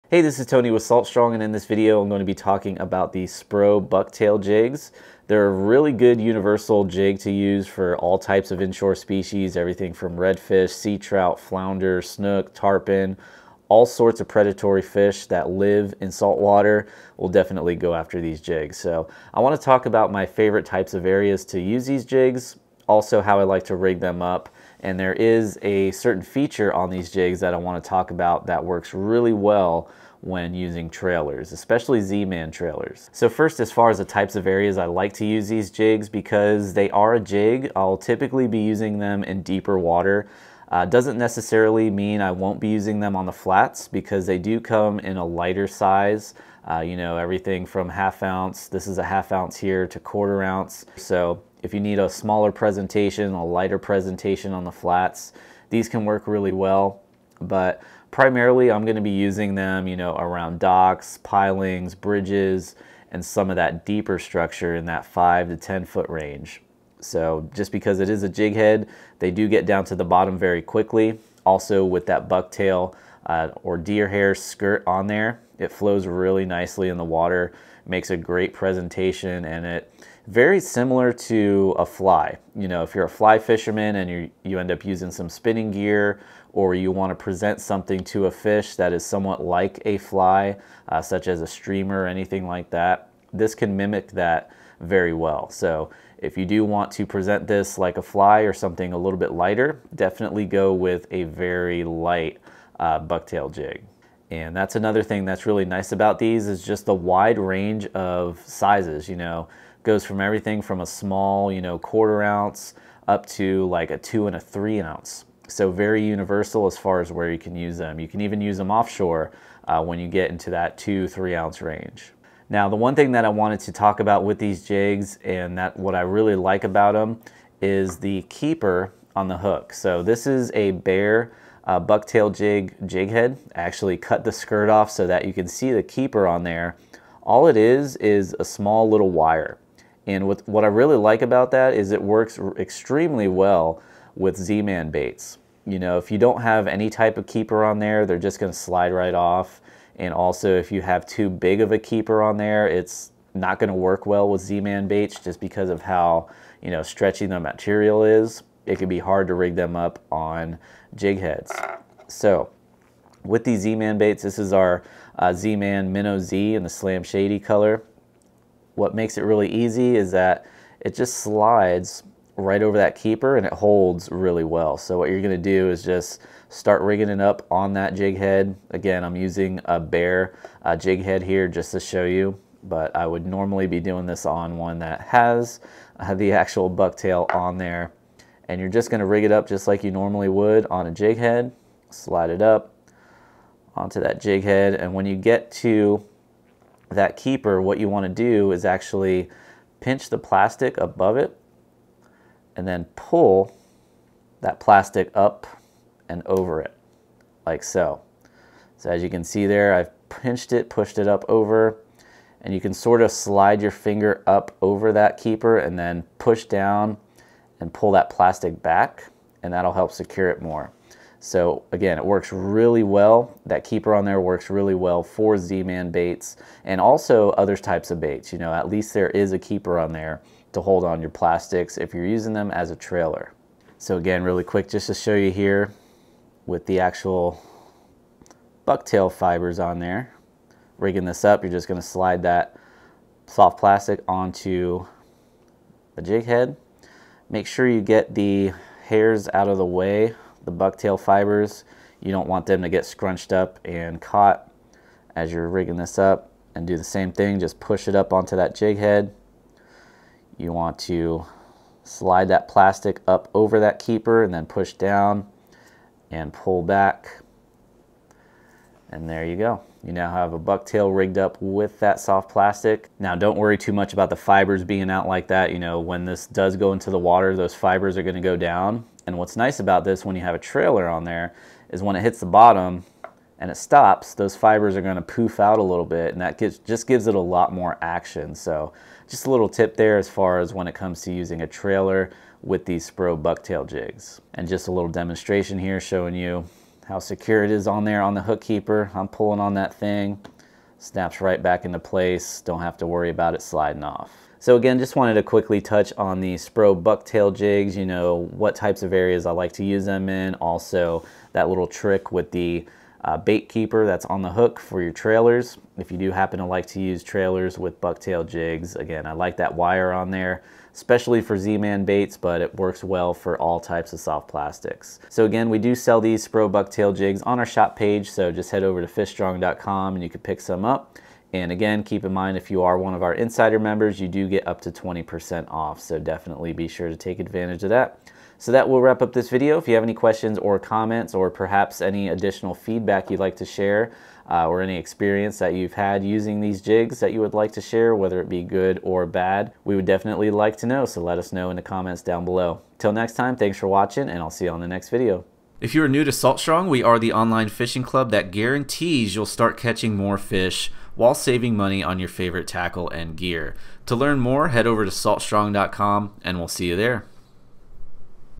Spro Bucktail Jig Review (Where To Use Them & How To Rig Them)
Stop paying full price for your tackle and get on the water fishing tips every week.

It's bucktail jig time!

Bucktail jigs are one of the most proven and popular lures.

In World War 2, the U.S. Navy even included a bucktail jig and a handline in the soldiers' survival kits so that they could catch fish if they needed to feed themselves.

Isn't that awesome?

Bucktail jigs are still really effective today and in this video, you'll learn all about the Spro bucktail jig, including:

- Where to use them
- How to rig them
- And a certain feature I especially like about them when using trailers

Have any questions about these jigs?

Have you used them before?